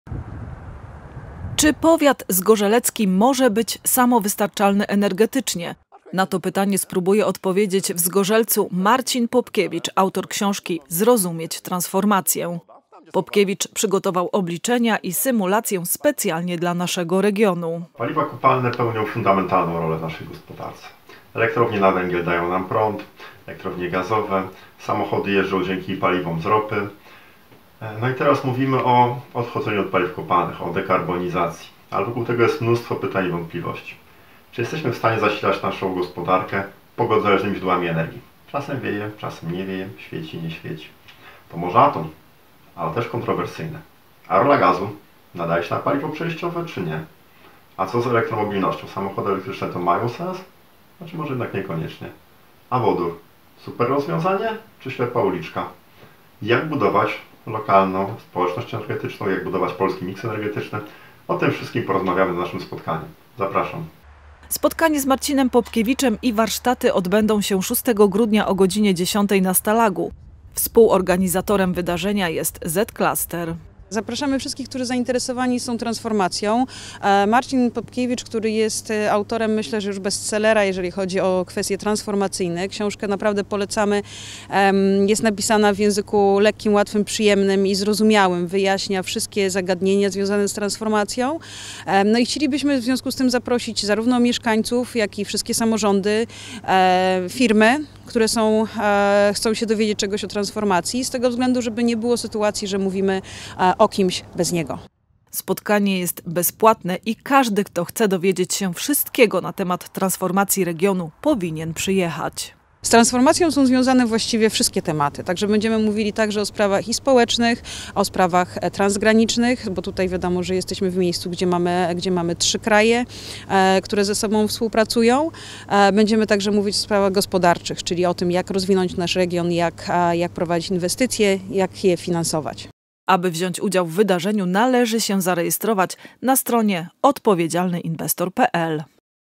Powiat samowystarczalny energetycznie?
Czy powiat zgorzelecki może być samowystarczalny energetycznie? Na to pytanie spróbuje odpowiedzieć Marcin Popkiewicz  autor książki „Zrozumieć transformację”.